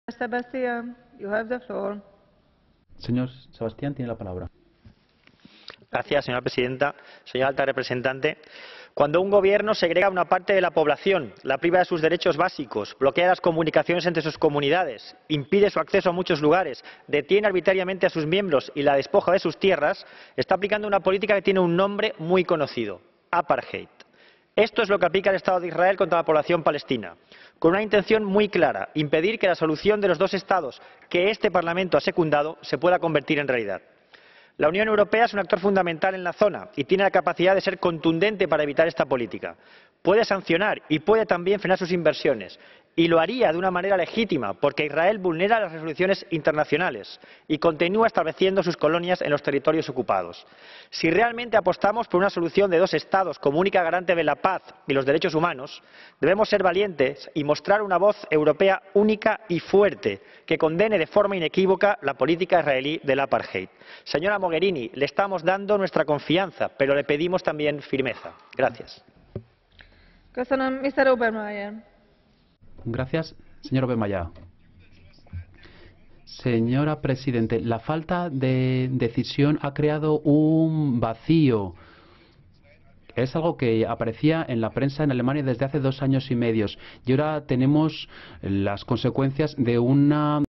Sebastià: "Israel aplica el apartheid contra la población palestina"
L'eurodiputat de Compromís-Primavera Europea defensa els drets dels palestins contra la política d'apartheid que practica l'Estat d'Israel.